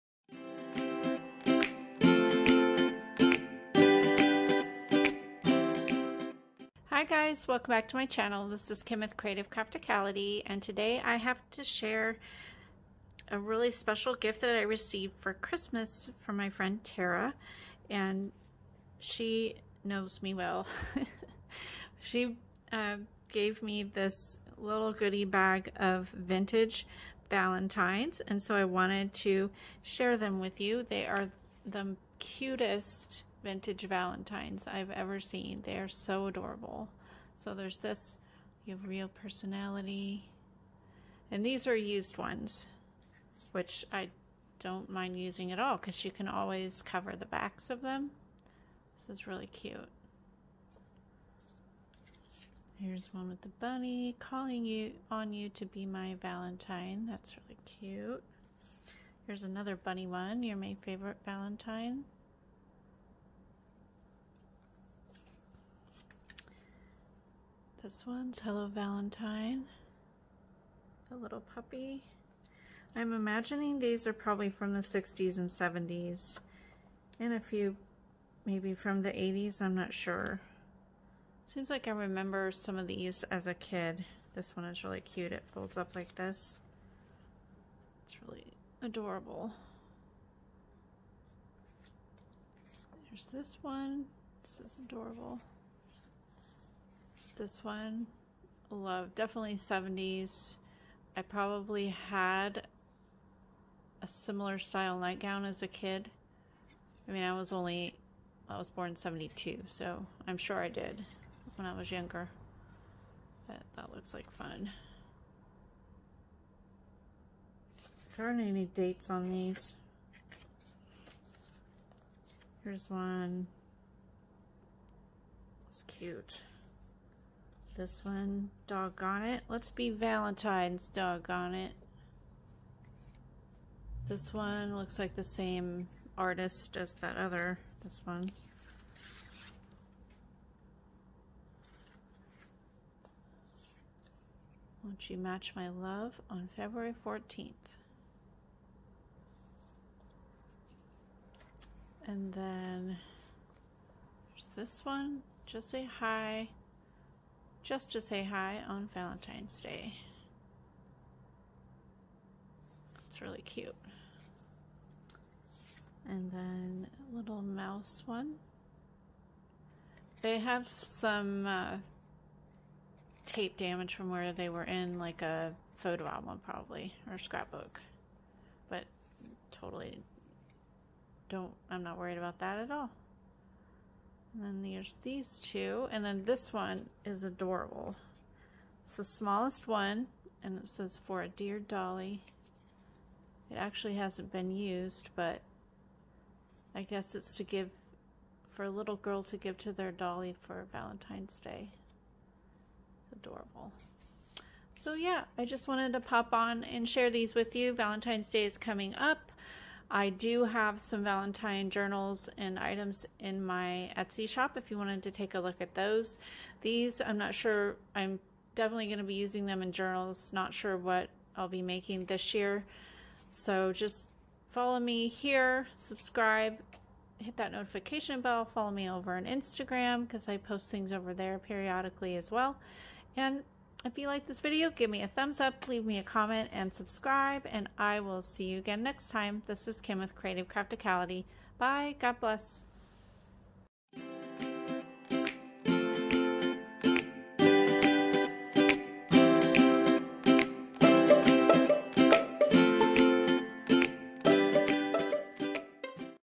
Vintage Valentines: A Gift from a Friend
Today I am sharing the vintage valentine's that I received for Christmas from a friend. I think they are so beautiful and fun!